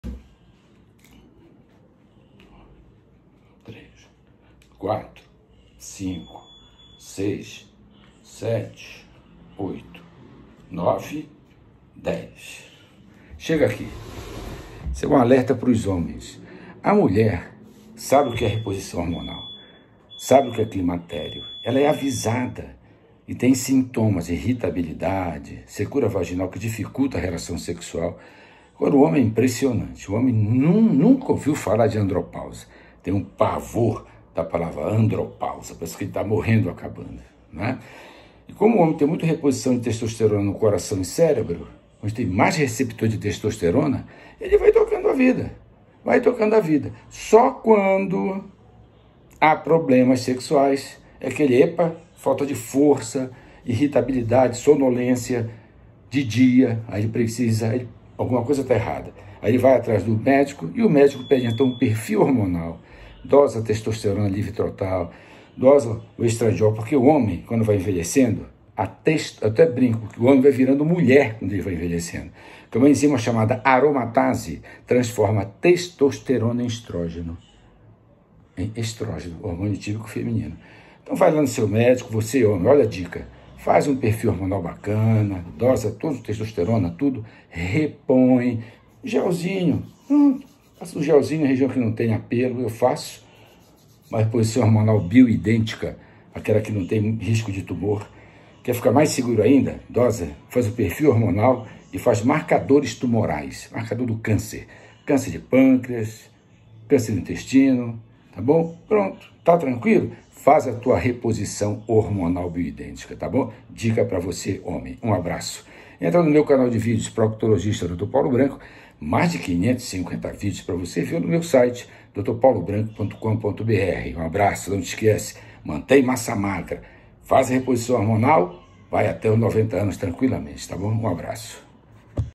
Longevidade: Dicas viver mais e com qualidade neste vídeo médico proctologista Dr Paulo Branco.
Médico da a dicas para os homens viverem mais e com qualidade de vida pela reposição hormonal Bioidêntica:
Proctologista: Dr Paulo Branco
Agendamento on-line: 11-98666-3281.
YouTube: proctologista dr paulo branco